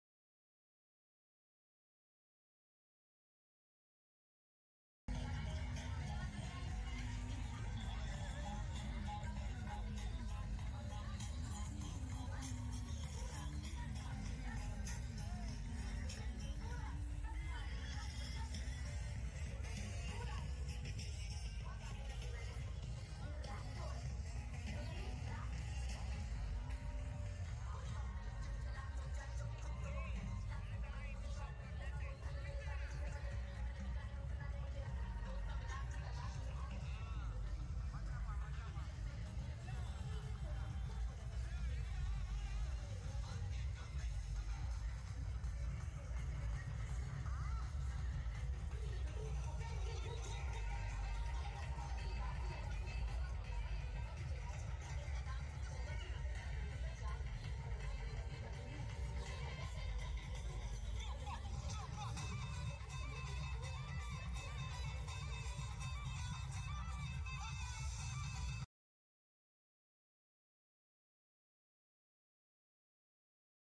Most crowded beach in Goa | Baga Beach | Goa Night Life December 31 night 2023
INDIA Most crowded beach in Goa | Baga Beach | Goa Night Life December 31 night 2023
Hello friends, This is First video of our Goa series 2023, In this video we are going to visit serene beaches  like Baga beach ,Anjuna and Calangute.
we also show you nightlife of baga and Tito's lane and LasOLas